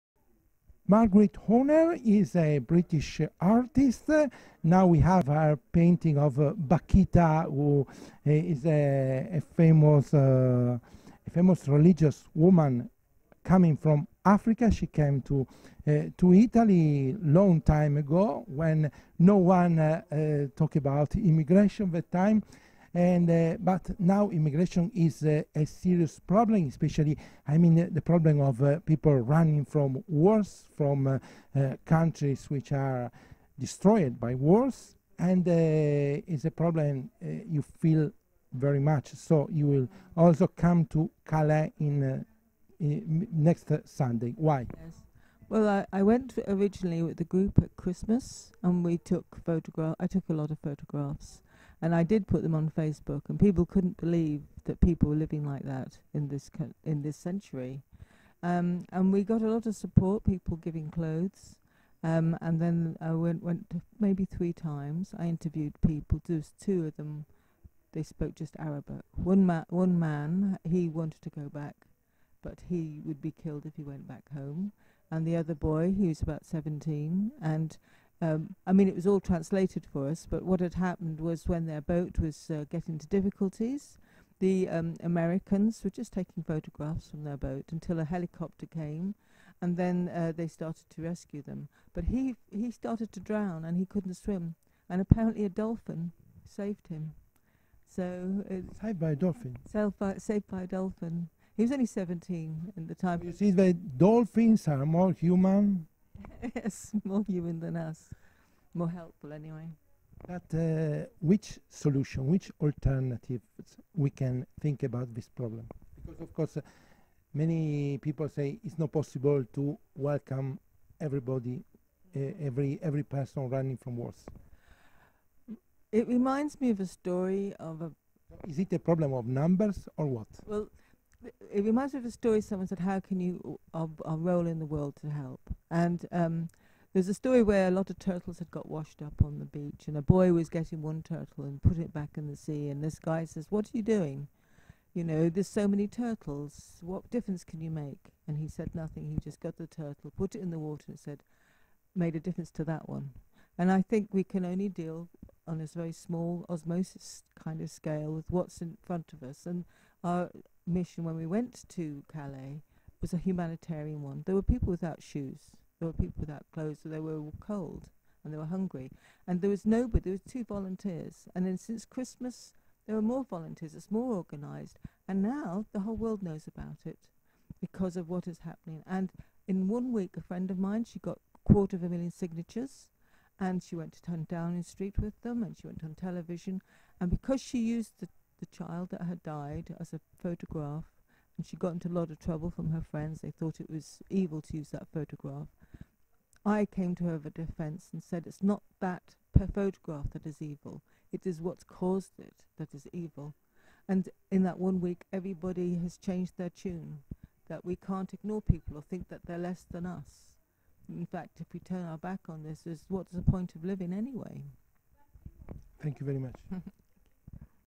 Refugees drama: the story of a teenager saved by a dolphin...
Marguerite Horner, a British artist partecipating to Comboni group mission, tells a touching story heard in Calais.